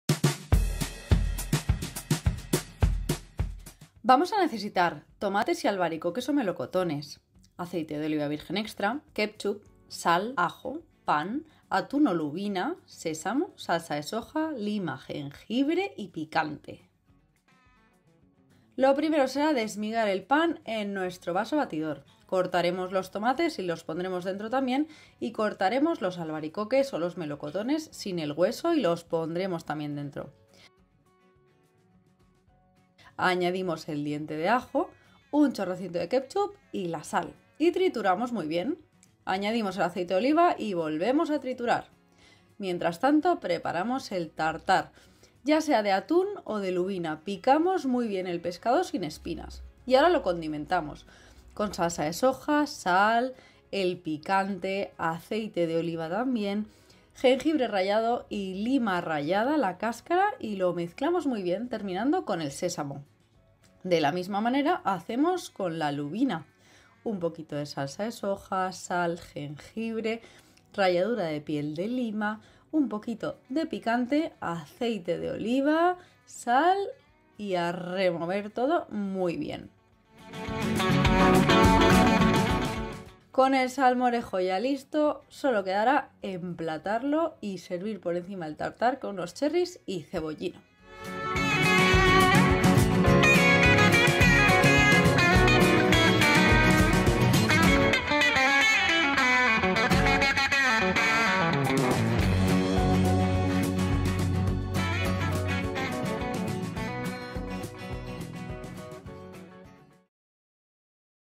Salmorejo de albaricoque con tartar de lubina o de atún | Lupa Supermercados
El salmorejo es un plato delicioso y muy refrescante que no puede fallar en tu mesa cuando llega el verano. Acompañado de un tartar de lubina o de atún, sorprenderá a tus invitados como un entrante fresco y jugoso.

¿Quieres aprender a elaborarlo? ¡Te dejamos la receta!

Ingredientes para el salmorejo:

2 tomates maduros con sabor
4 albaricoques o 2 melocotones
1/2 diente de ajo
1 cucharada de kétchup
60 g de pan duro
Aceite de oliva virgen extra
Sal
4 tomates cherry

Ingredientes para el tartar de lubina o de atún:

300gr de lomo de atún rojo o de lubina sin piel ni espinas
½ cucharadita de jengibre rallado
Salsa picante al gusto
1 cucharada de aceite de oliva virgen extra
Ralladura de piel de lima
1 cucharadita de salsa de soja
Sésamo
Sal

Troceamos el pan, el tomate y los albaricoques o melocotones previamente lavados y los ponemos en el vaso triturador.
Añadimos el ajo pelado, el kétchup y la sal y trituramos todo junto muy bien. A esto, incorporamos el aceite de oliva y volvemos a dejar triturando hasta que emulsione bien y no tenga grumos.

Picamos en pequeños trozos la lubina o el atún con el cuchillo y condimentamos con la salsa picante, el jengibre y la cáscara de lima rallados, la salsa de soja, el aceite de oliva, la sal y el sésamo. Mezclamos todo bien.

Servimos el salmorejo bien frío,  con la cantidad que queramos del tartar por encima, junto a unos trocitos de tomate cherry y cebollino picado como opción para decorar.

¡Seguro que te encantará!

Lupa, Tus vecinos de confianza.